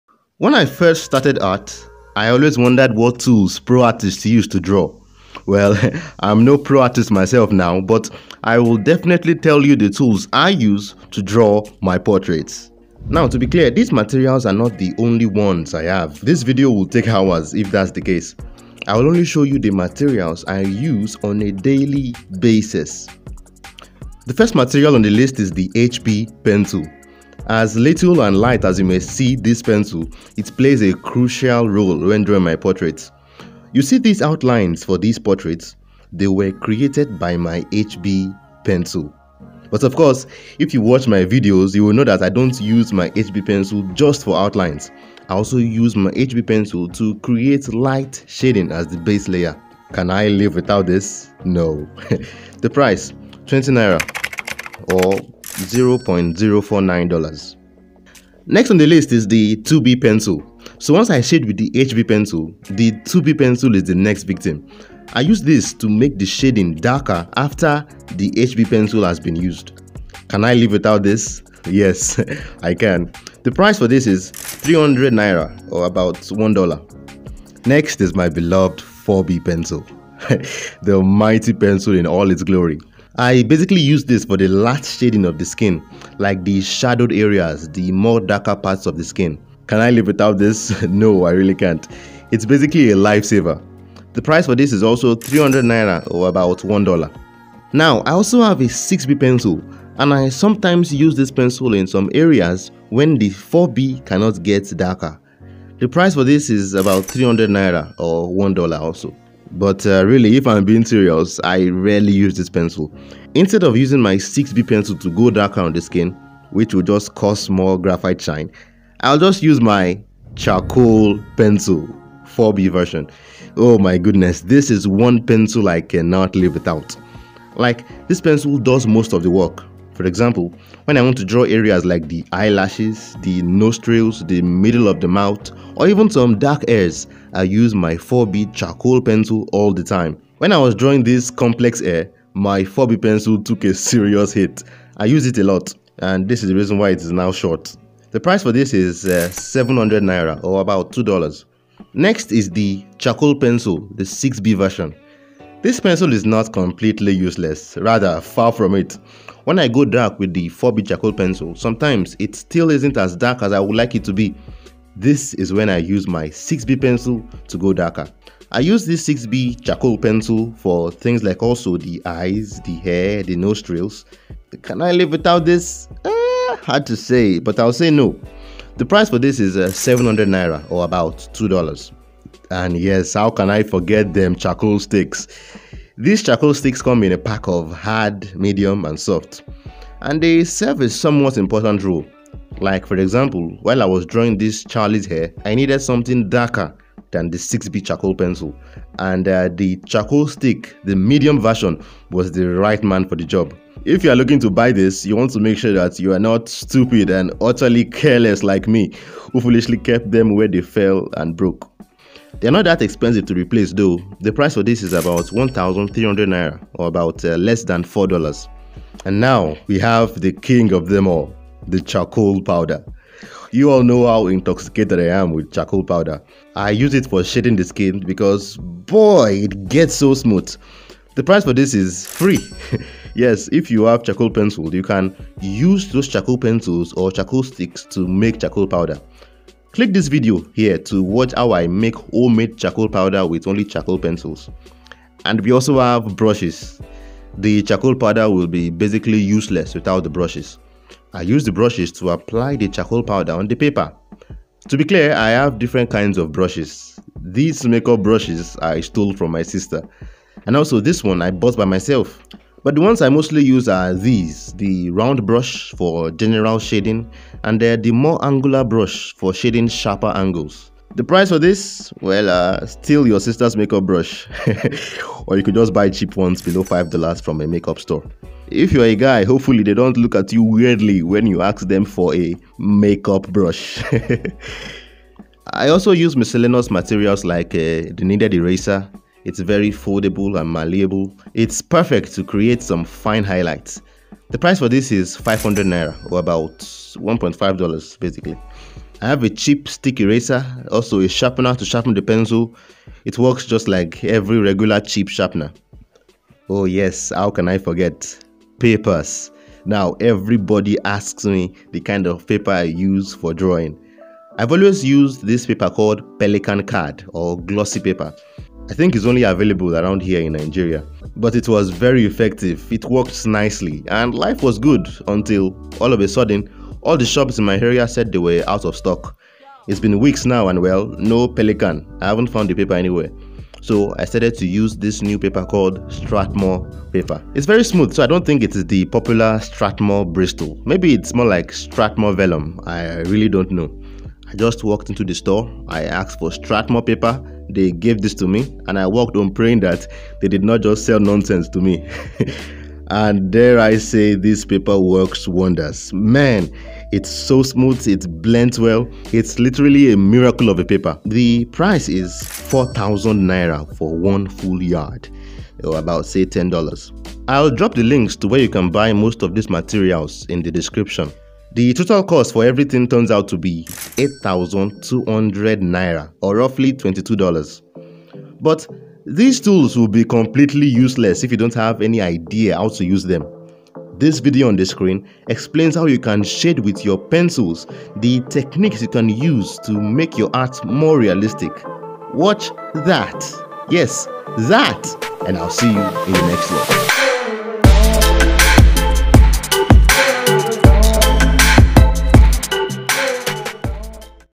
BEST ART SUPPLIES FOR REALISTIC DRAWINGS
This video talks about the best drawing materials for realistic drawings. I'll tell you all the supplies I use daily when I create realistic drawings. Many of these are must have art supplies for beginners. But while these contain drawing materials for beginners, they can also be used by professionals.

LINKS

Graphite Pencil set

Charcoal Pencil 4B and 6B

Kneaded Eraser

Charcoal Sticks HARD, MEDIUM and SOFT

Strathmore Bristol Smooth

Don't want homemade Charcoal Powder?

Apparently, it's not possible to buy pencils individually on Amazon, you might want to check your local Art Store if you want the pencils sold independently. I was able to find the Pencil set which I put links to above.

Let's take this further and be friends on social. Sounds good?

MUSIC CREDITS